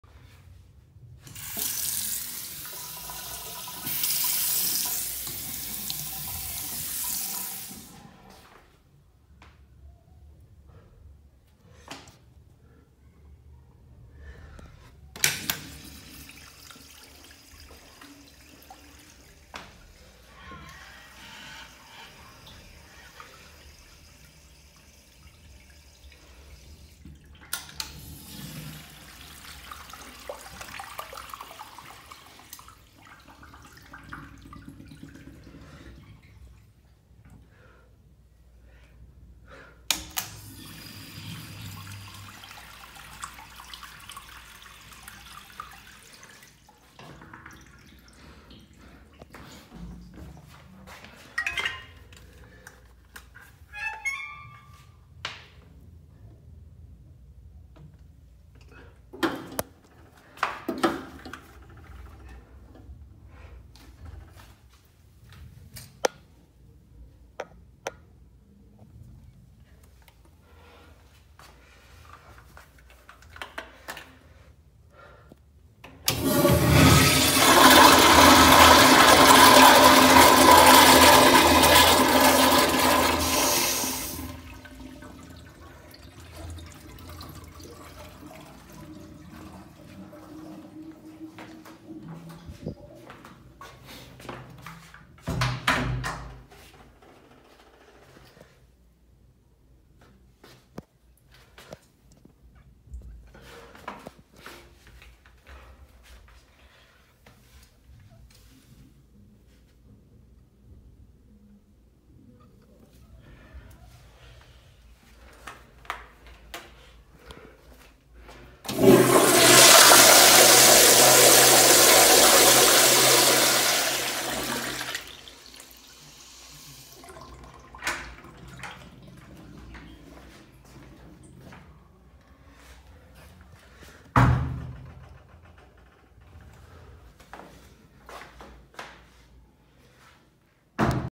788: Kohler Fixtures at a Community Center
Fixtures:
3 1992 Kohler Bardon Urinals
1 2010 Kohler Kingston Toilet
1 2018 Kohler Kingston Toilet
These Toilets replaced 1992 Kohler Kingston WaterGuard Toilets